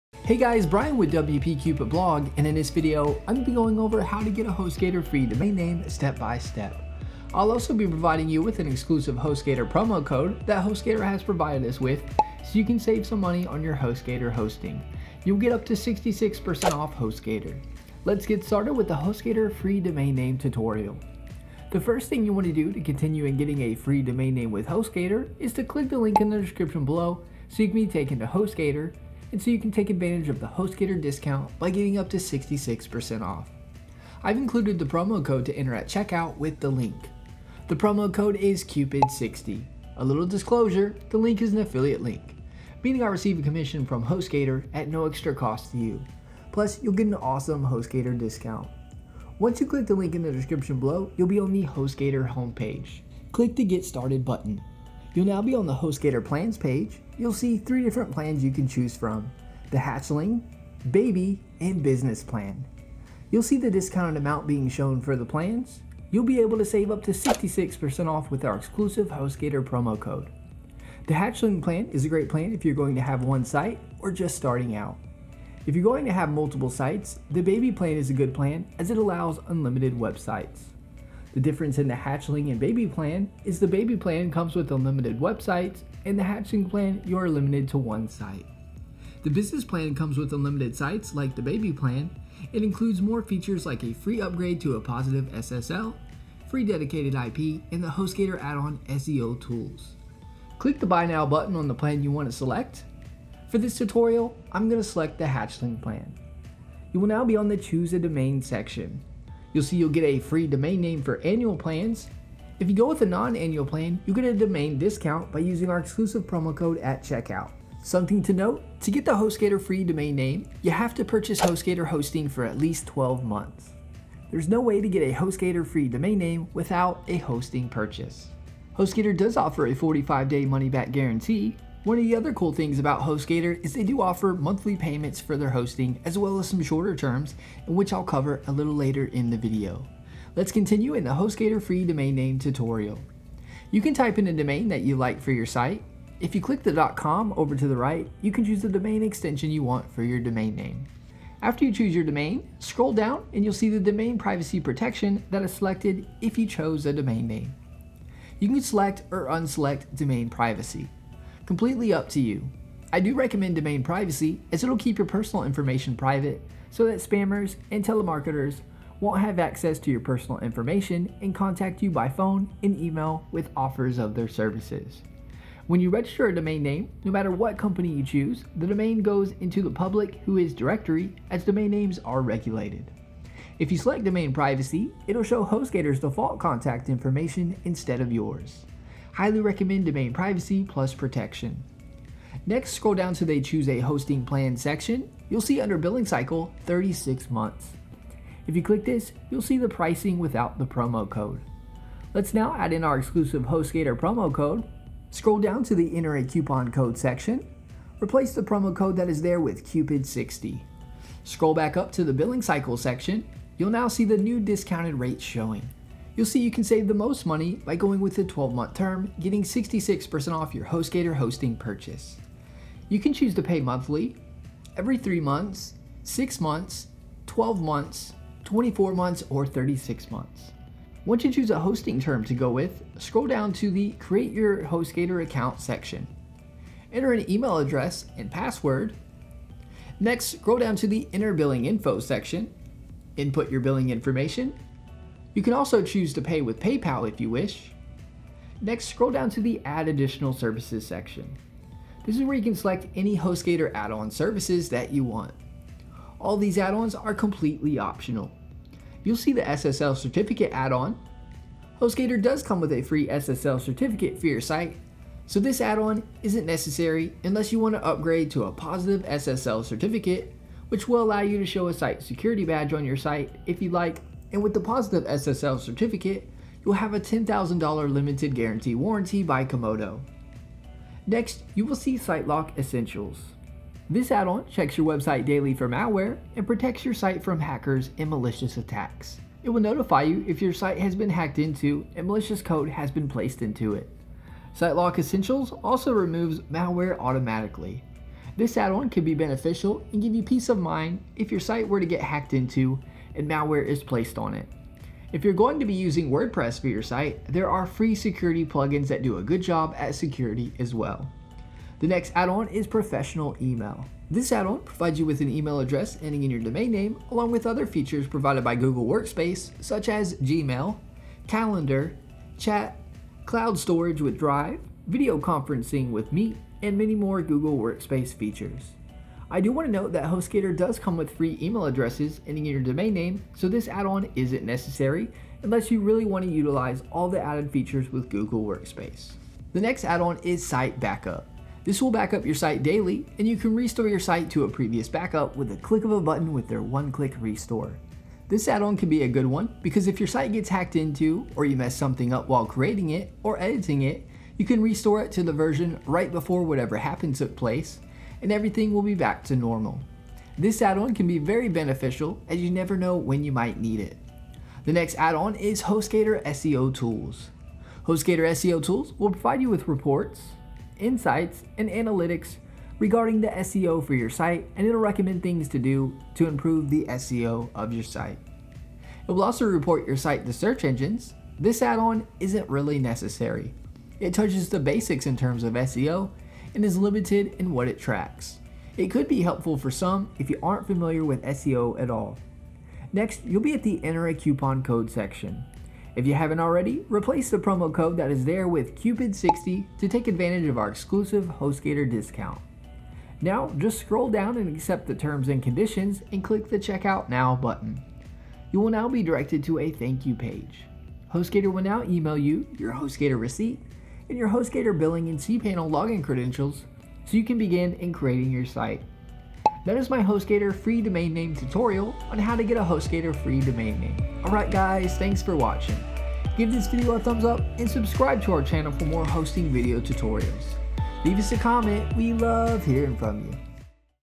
How To Get Hostgator Free Domain Name | Hostgator Tutorial
In this Hostgator free domain name tutorial, we go over how to get a free domain name with Hostgator step by step. This is a quick and easy Hostgator Free Domain Name tutorial to follow along with.

Getting a Hostgator free domain name consists of a few steps. Here are the steps to get a Hostgator free domain name in which we go over in the video:

Step one is to sign up with Hostgator.
Step two is to choose a hosting plan.
Step three is to choose a domain name.
Step four is to choose the hosting term.
Step five is to choose which Hostgator add-on services you'd like.
Step six is to complete payment.

To get the Hostgator free domain name you need to purchase Hostgator hosting. Hostgator doesn't give out a free domain name without purchasing their hosting.

To receive the free domain name you need to purchase hosting for a 12- month, 24-month, or 36-month term.